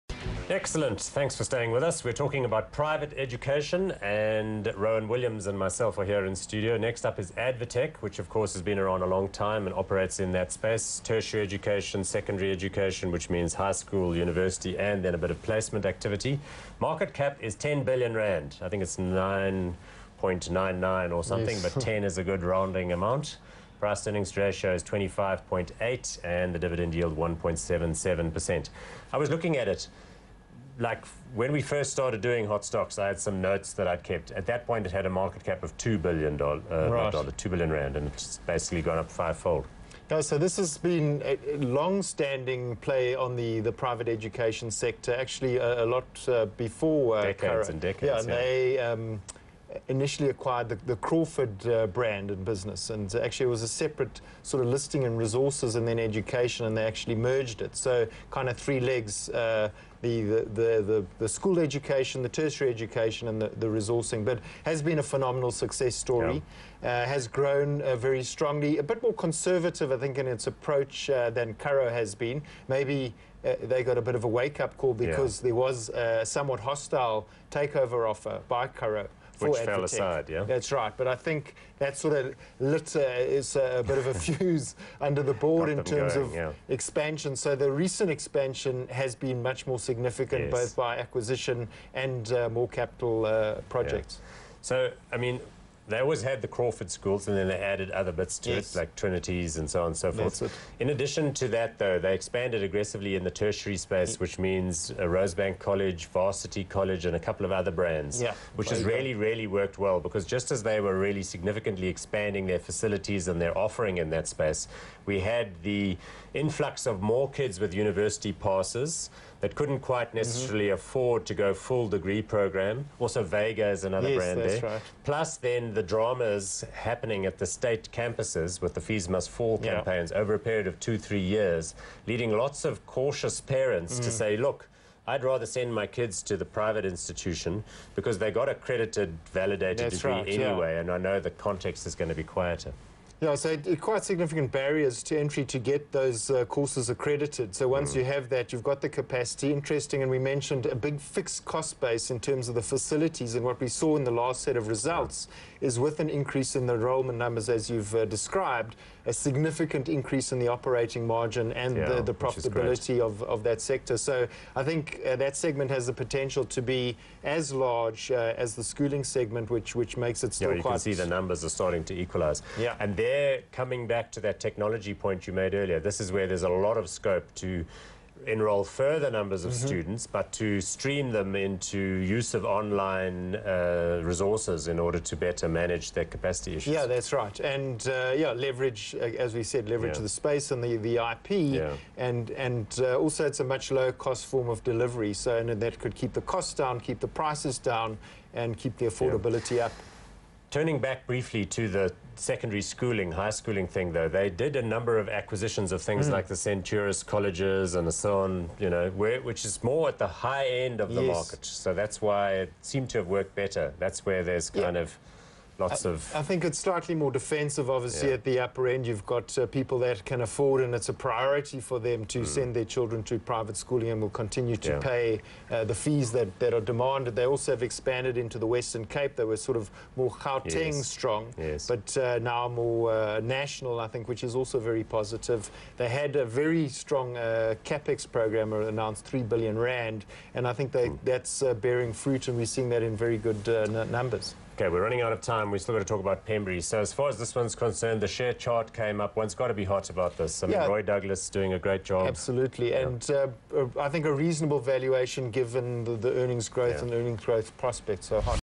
2017 06 05 CNBC Africa Hot Stocks Advtech 20h20
Rowan is a Director at Nitrogen Fund Managers and he talks about the market performance of ADvTECH with relation to its trading share price and growth.